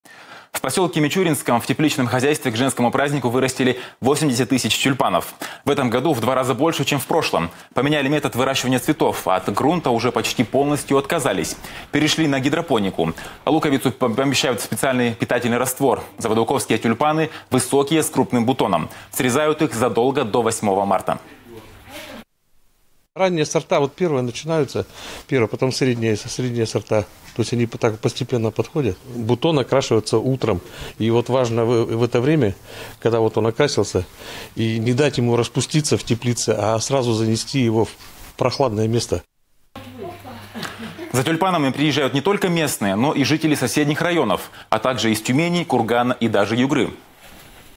Заводоуковские тюльпаны
В посёлке Мичуринском в тепличном хозяйстве к женскому празднику вырастили 80 тысяч тюльпанов. В этом году в два раза больше, чем в прошлом. Поменяли метод выращивания цветов. От грунта уже почти полностью отказались. Перешли на гидропонику. Луковицу помещают в специальный питательный раствор. Заводоуковские тюльпаны высокие, с крупным бутоном. Срезают их задолго до 8 Марта.